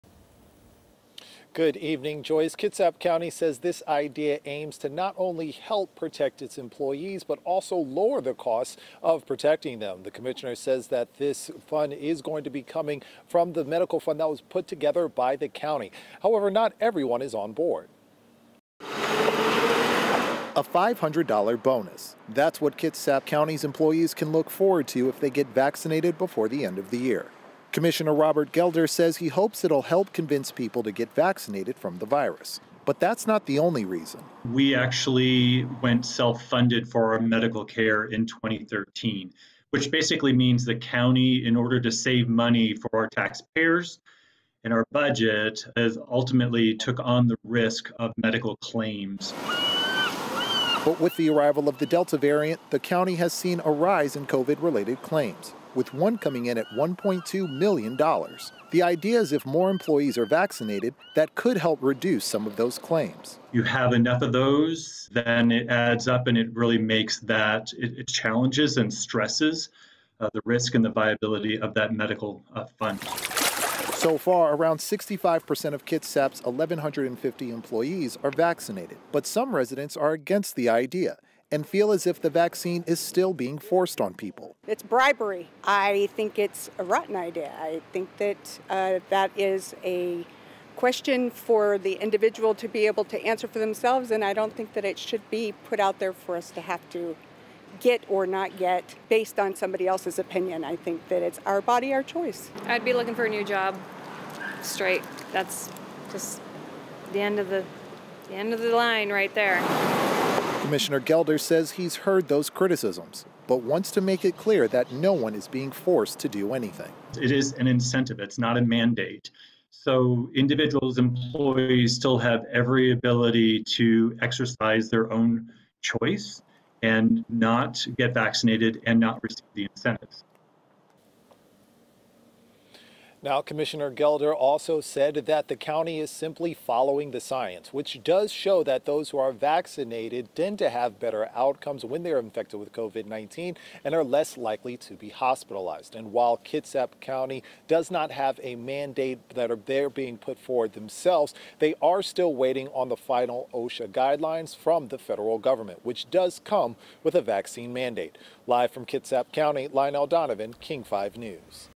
Kitsap County offers $500 COVID-19 vaccine incentive for its employees
Kitsap County is offering its employees through the end of the year a one-time payment of $500 for being vaccinated against COVID-19.

The Board of Commissioners unanimously passed the incentive on Monday.

Commissioner Robert Gelder said he hopes the incentive will help convince people to get vaccinated against the virus, but that’s not the only reason.